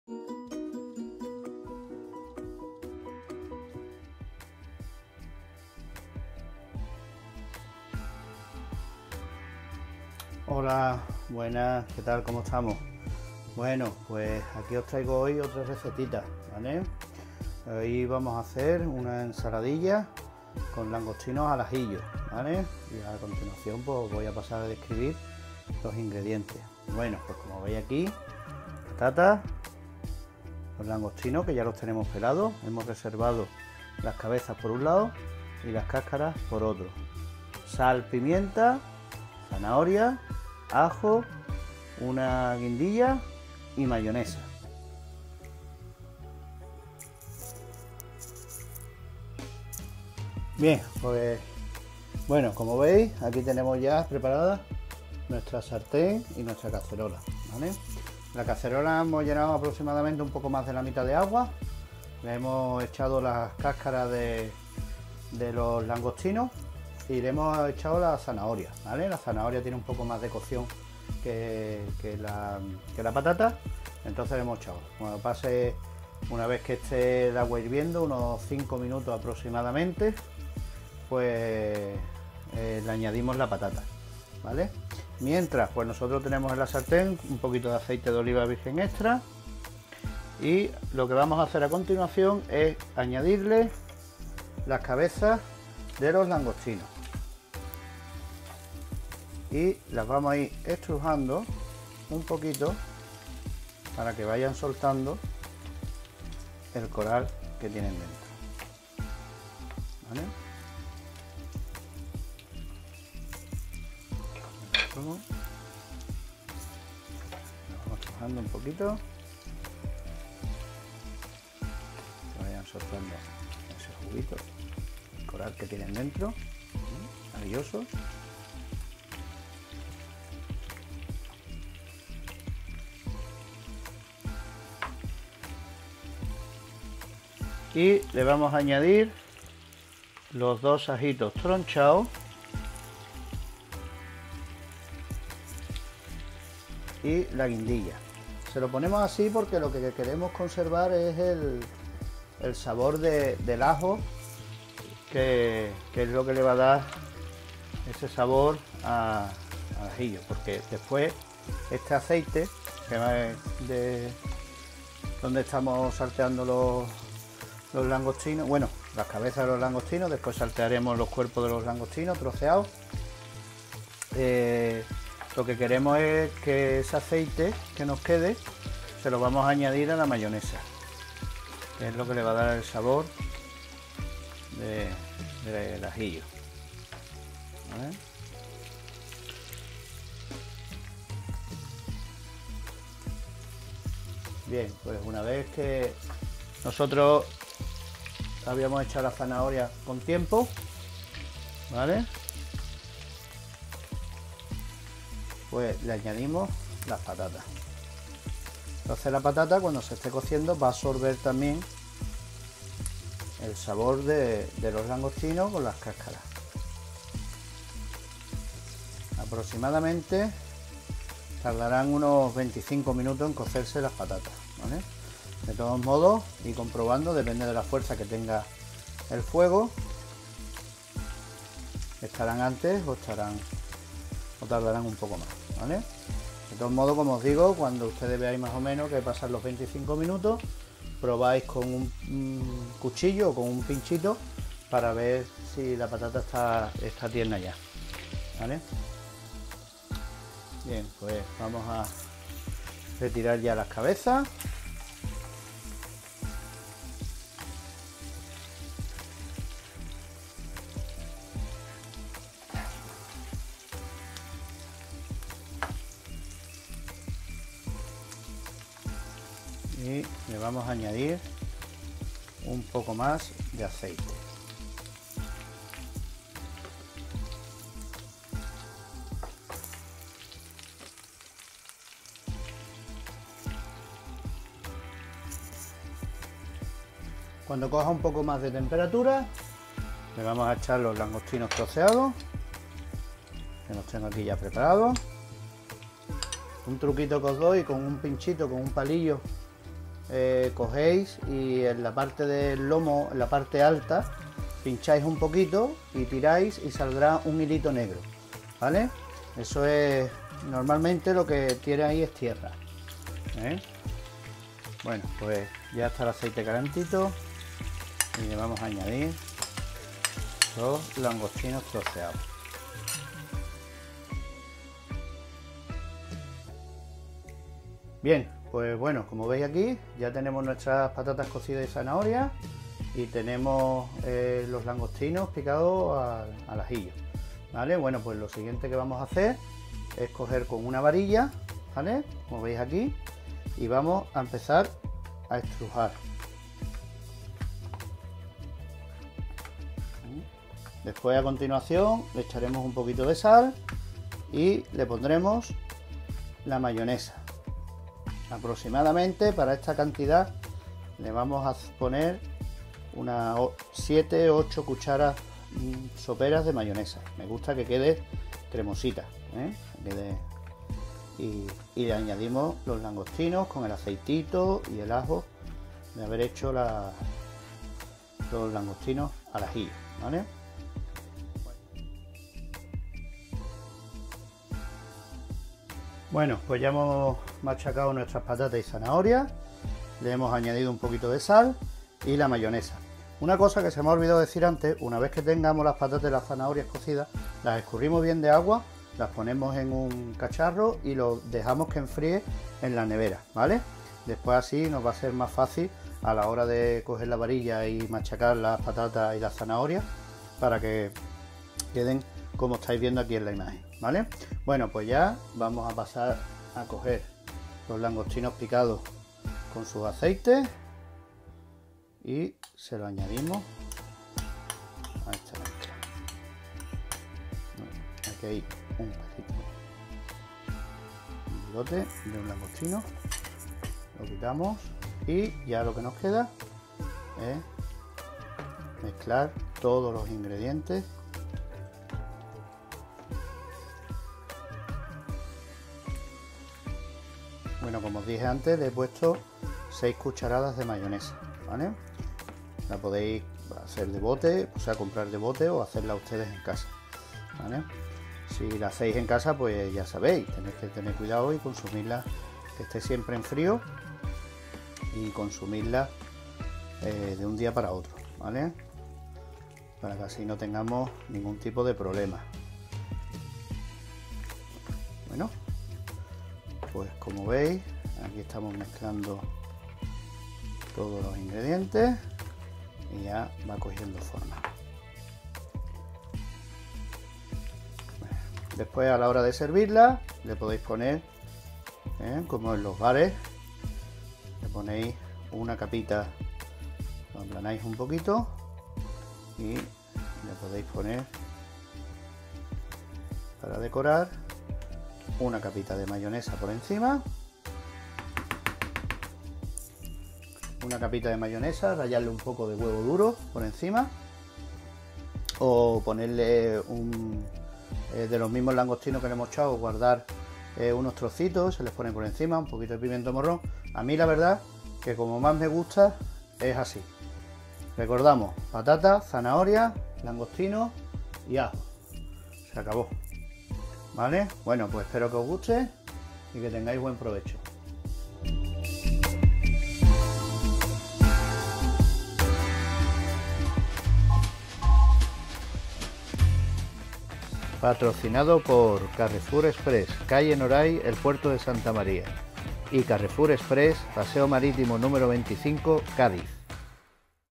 ENSALADILLA DE LANGOSTINOS AL AJILLO
Buenas a todos, hoy os traemos una receta muy fácil y muy recomendable para este tiempo veraniego en el que nos encontramos, aperitivo fresco que, acompañado de una copa, bien fría, de vino fino de jerez, como en otras recetas hay mil maneras de hacerlas y también hay infinidad de ingredientes que le podemos añadir a la ensaladilla, también en la receta os indico que utilicéis mayonesa de bote, sobre todo por el cuidado que hay que tener con los huevos en este tiempo, cuando hagáis el sofrito para los langostinos al ajillo recordar de estrujar bien las cabezas para que suelten todo su jugo y sabor y limpiar las tripas de los langostinos, también muy importante retirar la guindilla una vez que utilicemos el jugo del sofrito para añadírselo a nuestra mayonesa, espero que os guste y que tengáis buen provecho.

•Ingredientes:
Langostinos pelados (reservamos cascaras y cabezas)
Patata para cocer (Tipo Monalisa)
AOVE
Ajo
Zanahoria
Guindilla
Mayonesa de bote
Sal
Pimienta

•Utensilios:
Fuegos o vitroceramica
Cacerola
Sarten
Pelador
Fuente para servir
Boles
Varilla
Cuchillo cebollero
Tabla de corte